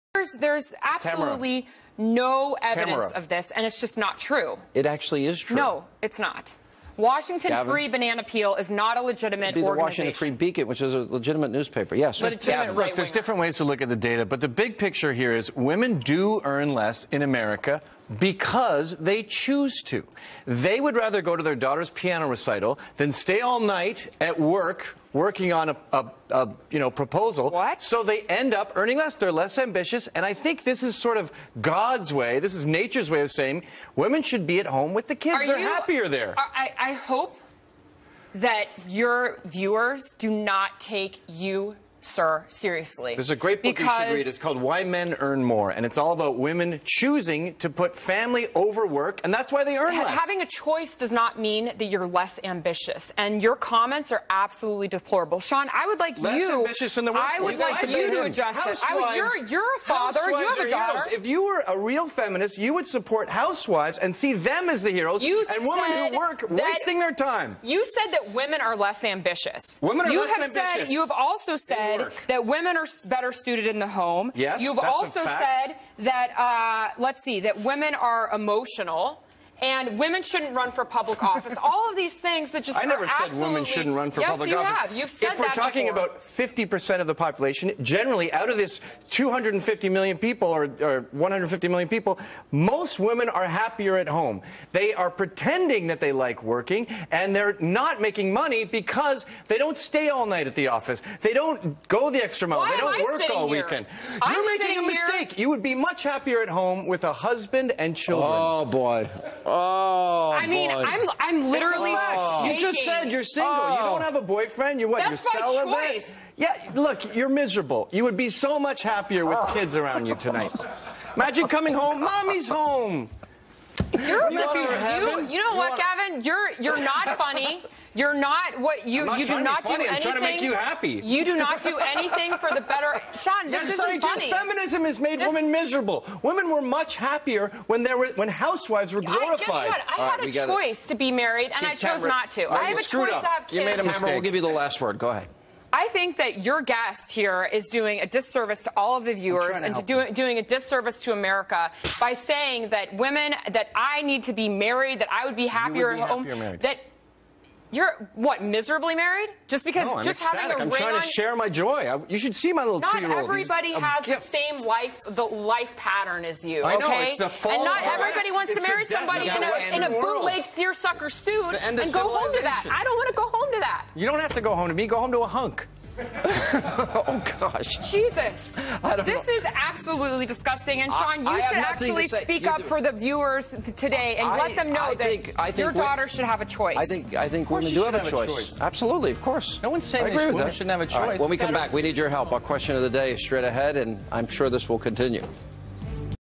Że niby kobiety są bardziej emocjonalne? | Feministka strollowana przez Gavina McInnesa
„Kobiety zarabiają mniej niż mężczyźni, bo są mniej ambitne”
Powiem tak - jesteś sfrustrowana i byłabyś szczęśliwsza spędzając ten wieczór ze swoimi dziećmi. Wyobraź sobie, że wracasz dziś do domu i te nie mogą się z radości na powrót mamy.

Źródło: Feminist Gets Trolled By Gavin McInnes Until She's Shaking With Rage

I have done subtitles for this video.
I do not own nor claim any rights to this content. I am publishing this solely for informational and educational purposes.